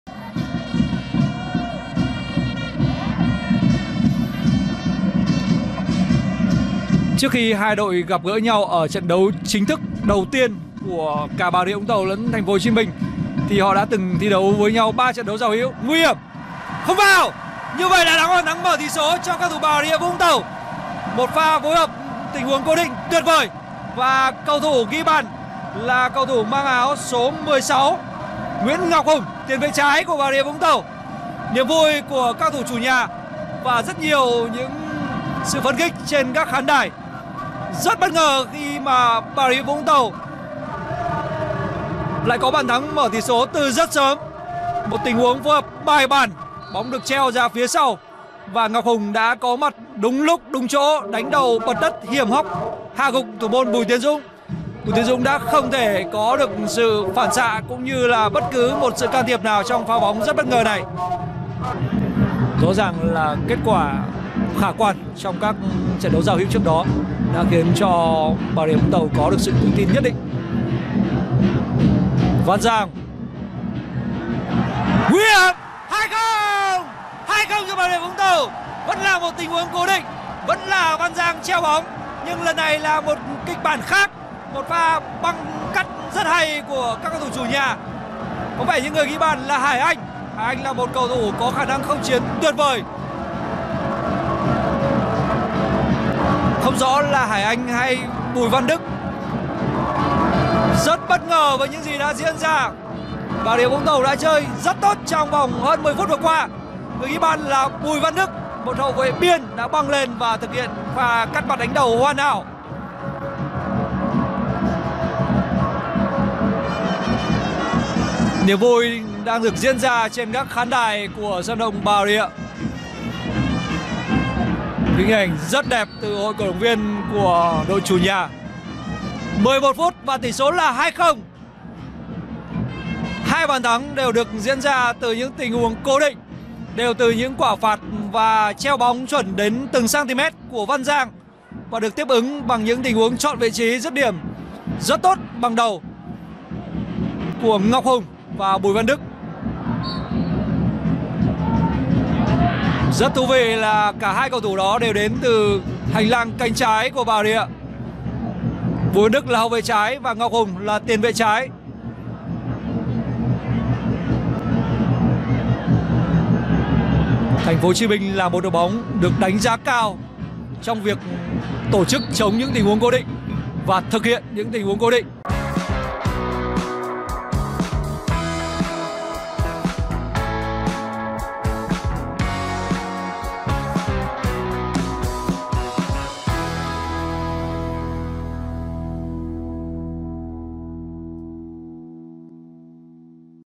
Toang...Bùi Tiến Dũng thủng lưới 2 bàn trong 10 phút | Bà Rịa Vũng Tàu vs CLB TP HCM
Lửa Bóng Đá chúc các bạn có những giây phút thư giãn vui vẻ.